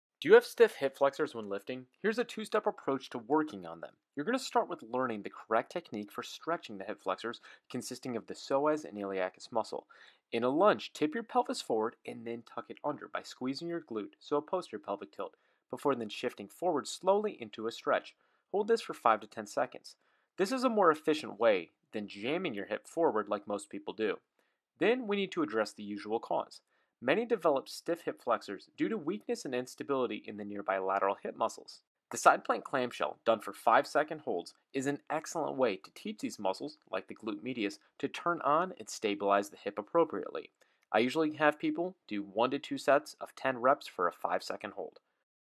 How to Fix Stiff Hip Flexors
Here's a 2 step approach to fixing tight hip flexors. First you need to learn how to stretch them correctly and second, you need to address WHY They often feel so stiff in the first place (usually as compensation due to weakness/instability within the lateral and posterior glutes).